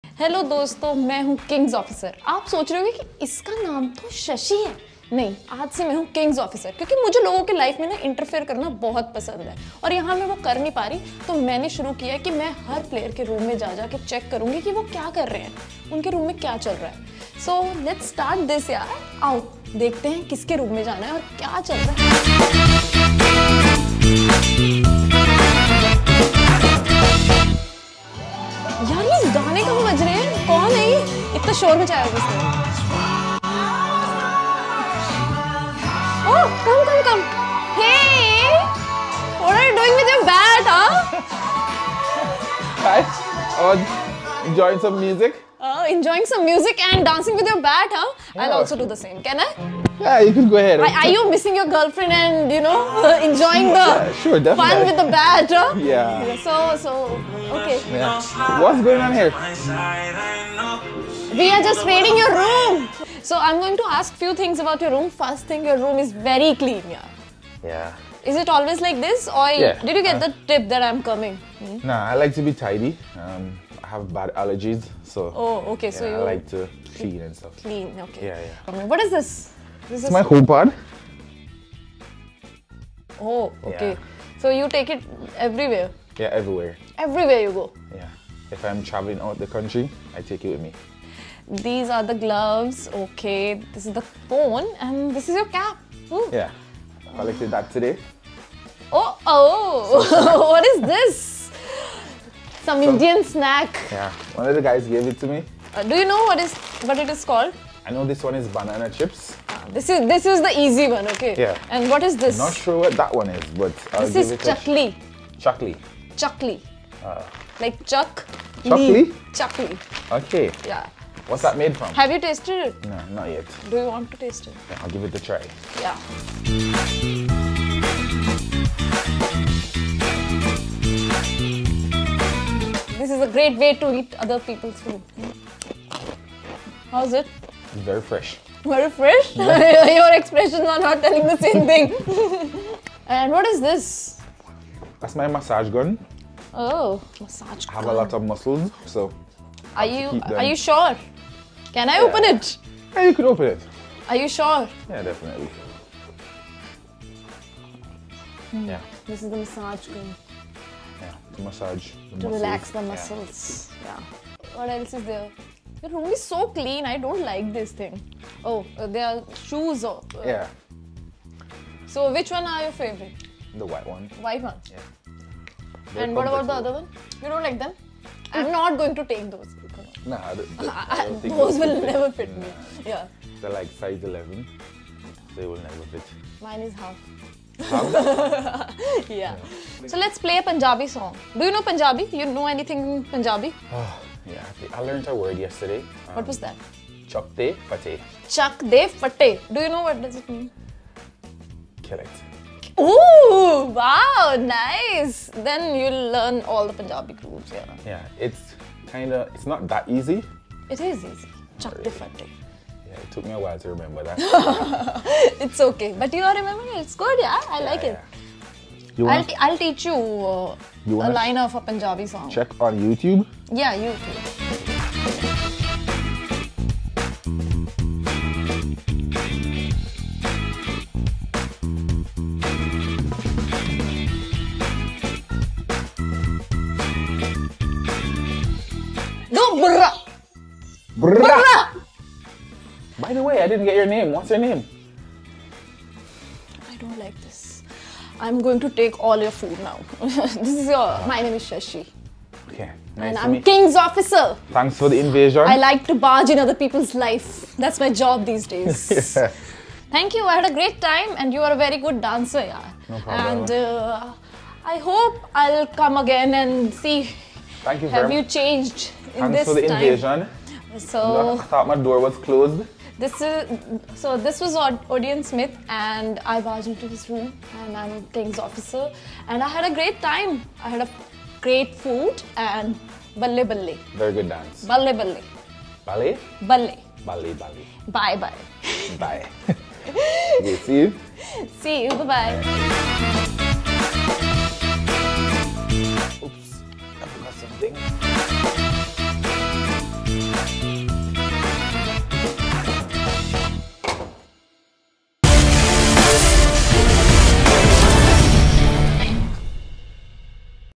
Kings' Officer Episode 1 | Shashi Raids Odean's Room | IPL 2022
Here’s presenting you with the first episode of Kings' Officer ft. Odean Smith. Watch to know the things our Kings' Officer Shashi Dhiman discovers when she raids the room of our Sher Odean Smith.